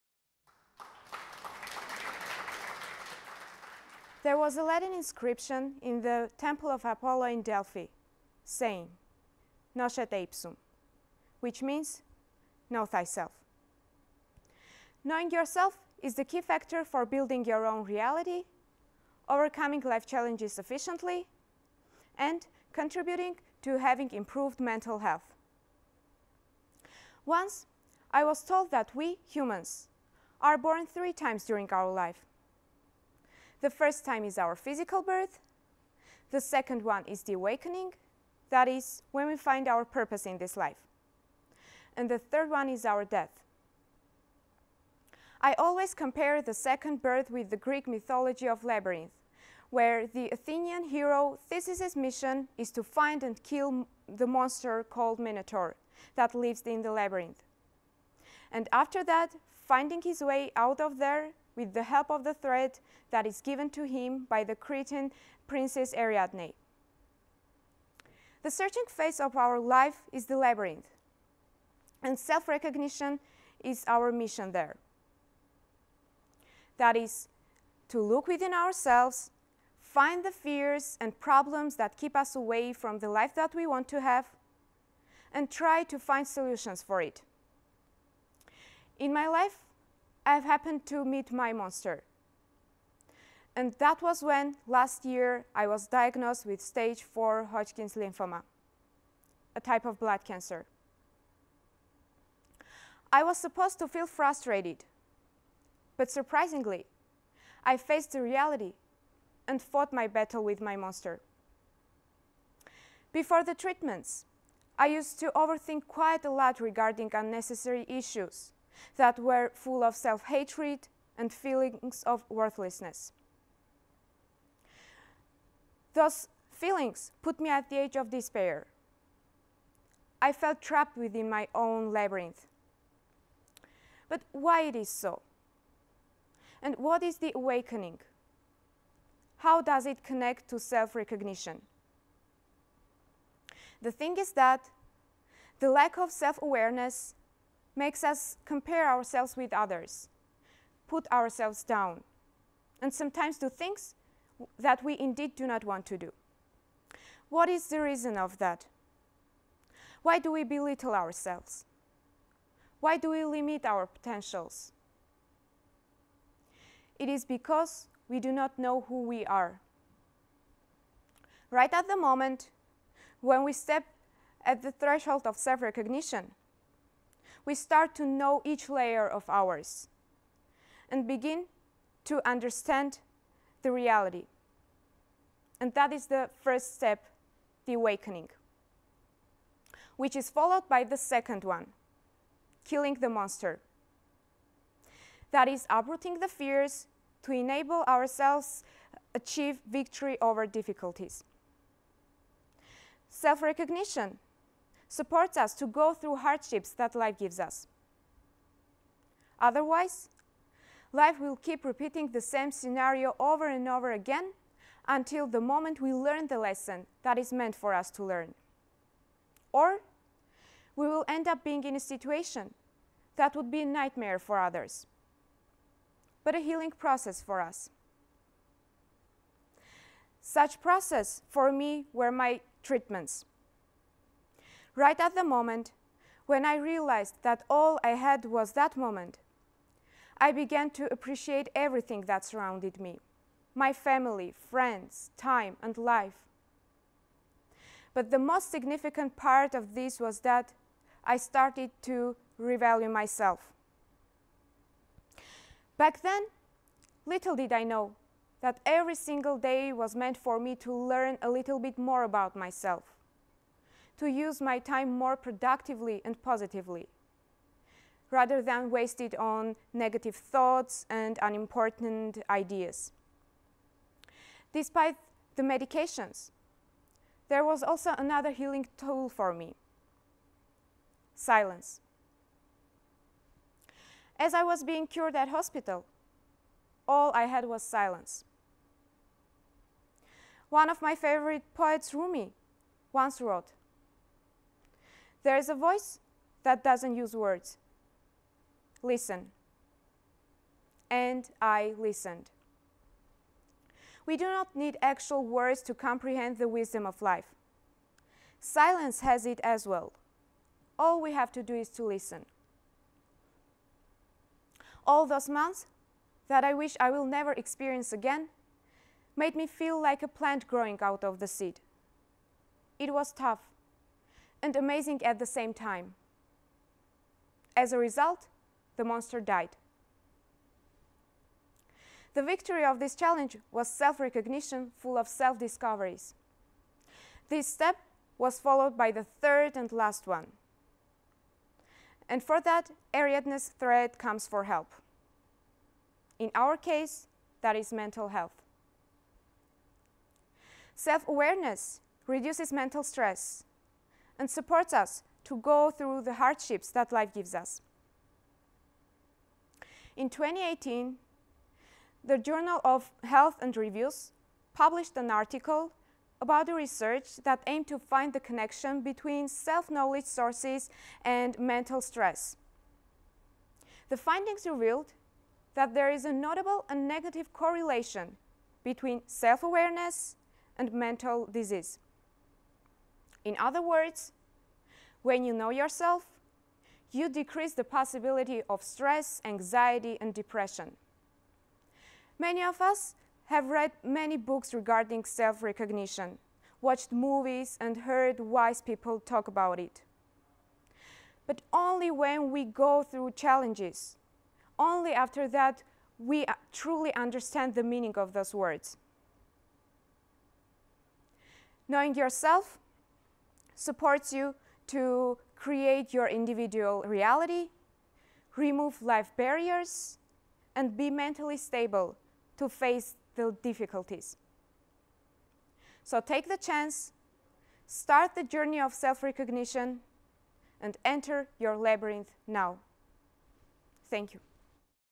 How to find your way out of the labyrinth of life | Gohar Geghamyan | TEDxAUA
Do you know yourself? In this talk, Gohar takes you on a journey through her life's labyrinth and shows you step by step how you can walk your path to self-discovery. Gohar is currently pursuing her Bachelor's degree in Politics and Governance. For a while, she volunteered as a journalist at “Champord” newspaper. She spends her free time reading novels and poems, and writing literary pieces. This passion led her to work at a bookstore before pursuing her education at AUA. Gohar is certain that almost every life barrier can be removed only if one is eager to discover their true potentials and inner power. And the cornerstone of that is self-recognition. This talk was given at a TEDx event using the TED conference format but independently organized by a local community.